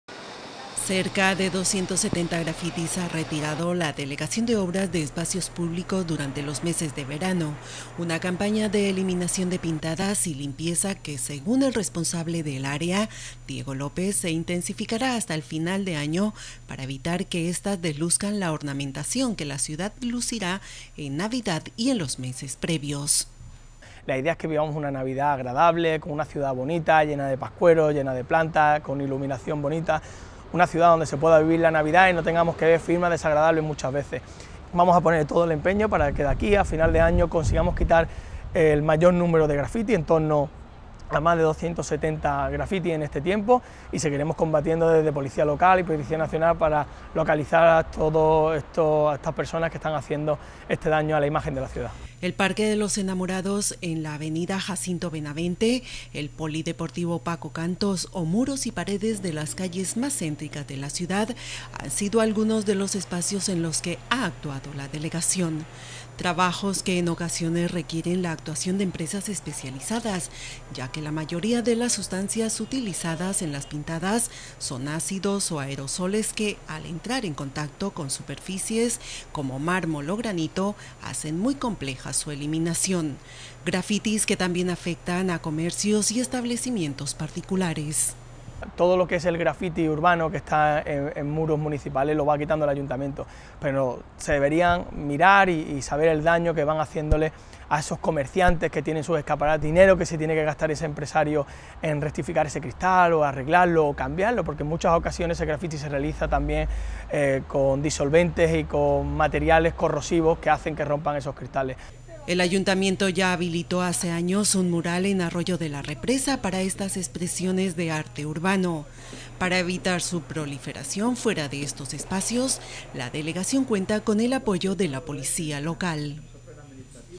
Se intensifica de cara a navidad la campaña municipal de limpieza de graffitis en espacios públicos
Durante el verano ya se han eliminado unas 270 pintadas y con el objetivo de mantener el ornato, especialmente en fechas señaladas, el consistorio va a intensificar una campaña que nunca ha abandonado. El concejal, Diego López, asegura que algunos de los graffitis son de muy compleja eliminación.